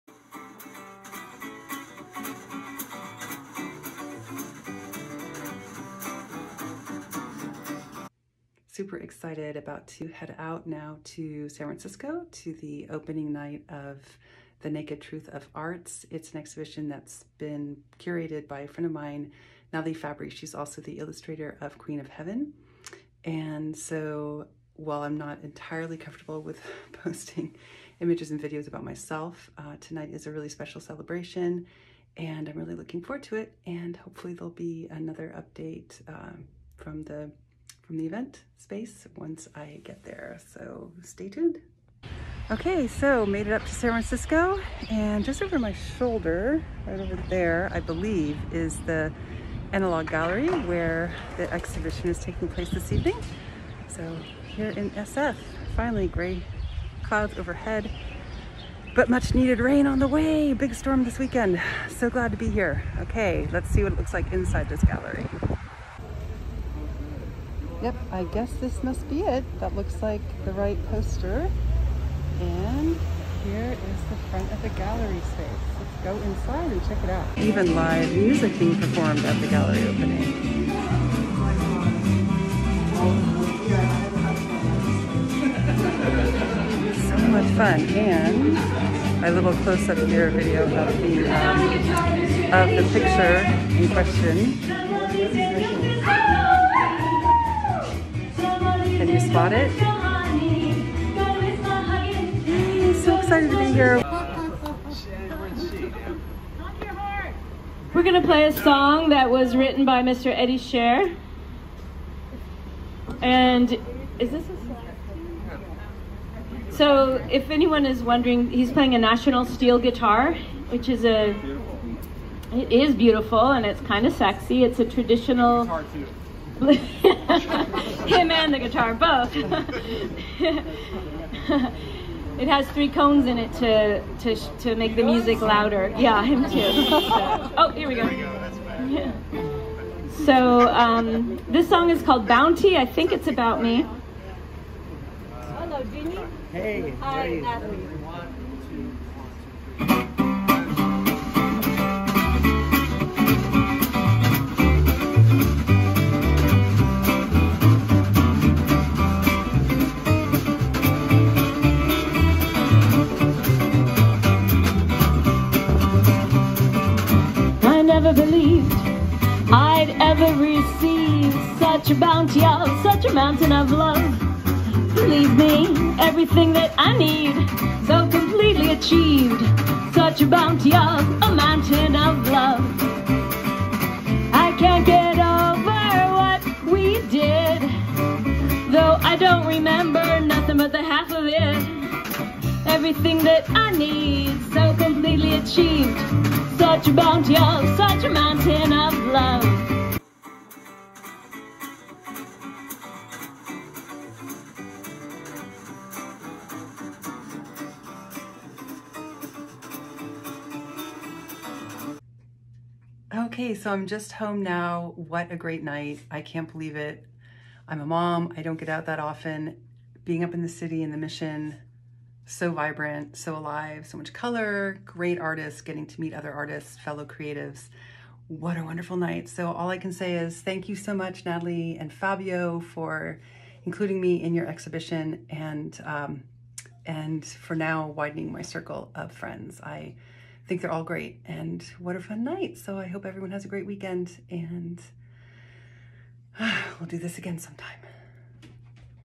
The Naked Truth of Arts Exhibit - Featuring The Templar's Garden by Catherine Clover
This is a film I shot  on the opening night of The Naked Truth of Arts - 50 Artists 50 Lives 50 Stories exhibited at the An.a.log gallery in the Mission District of San Francisco. Special thanks to Nathalie Fabri and Fabio Reis for including me and The Templar's Garden in the show!